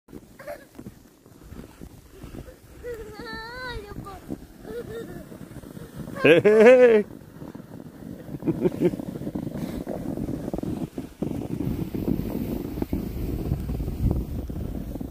Amish Girl Pulling Brother
6-year-old Lisa is pulling he brother Ryan on a sled with her new pony!